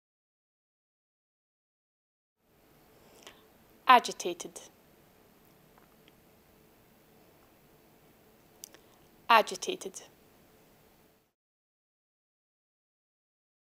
How to pronounce AGITATED in British English
This video shows you how to pronounce AGITATED in British English. Speaker has an accent from Greenock, Scotland. Collins Dictionary, the home of living English and pioneers of dictionary publishing